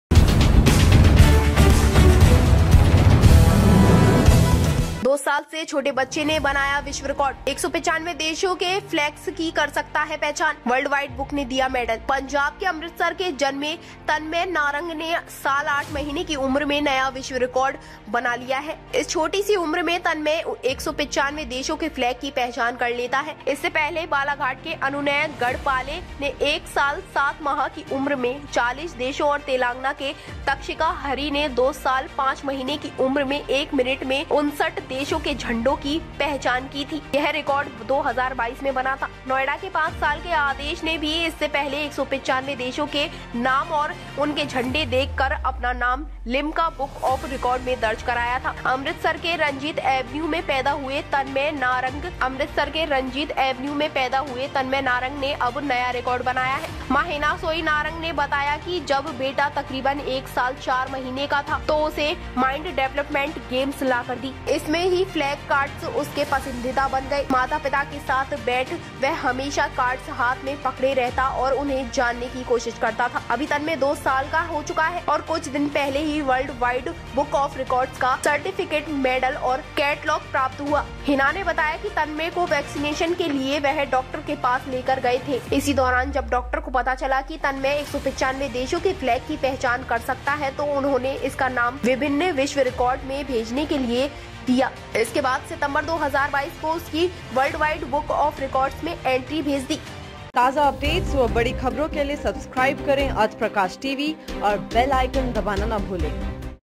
2 साल से छोटे बच्चे ने बनाया विश्व रिकॉर्ड
पंजाब के अमृतसर में जन्मे तन्मय नारंग ने 1 साल 8 महीने की उम्र में नया विश्व रिकॉर्ड बना लिया है। इस छोटी सी उम्र में तन्मय 195 देशों के फ्लैग की पहचान कर लेता है। इससे पहले बालाघाट के अनुनय गढ़पाले ने 1 साल 7 माह की उम्र में 40 देशों और तेलंगाना के तक्षिका हरि ने 2 साल 5 महीने की उम्र में एक मिनट में 69 देशों के झंडों की पहचान की थी। यह रिकॉर्ड 2022 में बना था